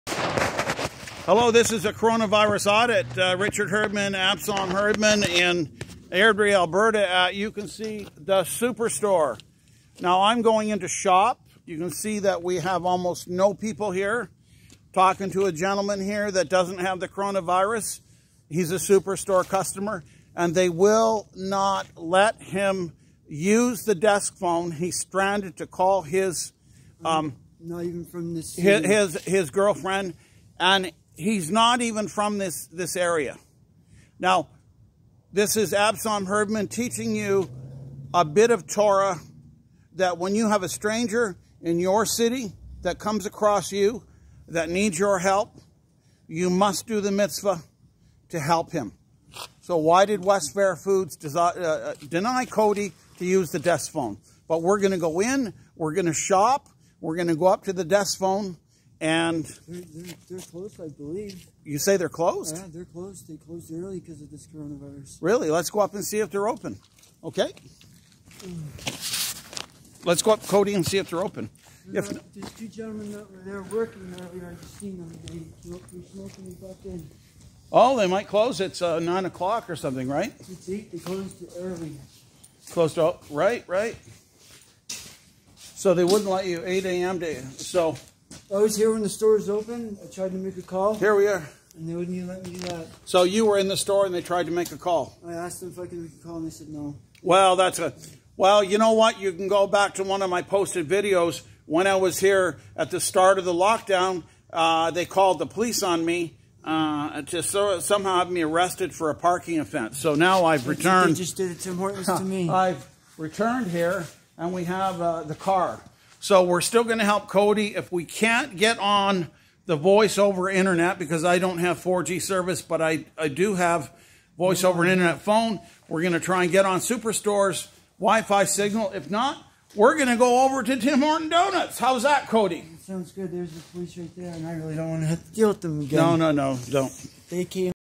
Airdrie ab VISITOR *CORONA VIRUS AUDIT*no one will help !Cold*Snow*PoorClothing 3april 8pm2020
20 yo will perish of exposure in SUPERSTORE AIRDRIE AB CANADA 3april8pm  2020 SHABBAT SHALOM 9nisan5780  , businesses and people refuse use of phone to stranded man or offer assissance OVER MANY HOURS to this lost lonely strung out  young man
SAD AND REPULSIVE . Herdman ascertains facts calls 911 then speaks directly to RCMP officer at TIM HORTONS DONUTS
.Cody is a mental health patient in need of immediate housing and compassion and stabilisatikn by a medical mental health team

absalom herdman was feeding his horses and on the way home dropped into superstore with his car and dogs ...for shabbat supplies and noticed this frantic abandoned man snow storm, cold , very thin clothes walking up to cars and business and facing rejection and distain A FATAL SITUATION UNFOLDING

Herdman has a wifi voice over internet phone and STOPPED THE SUPERSTORE SNOW SANDING TRUCK for assisstance, ordering him to call police on 911 AND THIS CONTRACTOR REFUSED , stunning

He related to me he was from nova scotia and 3 hours previous 2 people in a car drove him to Tim Hortons superstore airdrie, tricked him , sent him into Tims to get a coffee and drove off abandoning leaving him destitude

This was an urgent Criminal Code matter and medical emergency with Cody the victim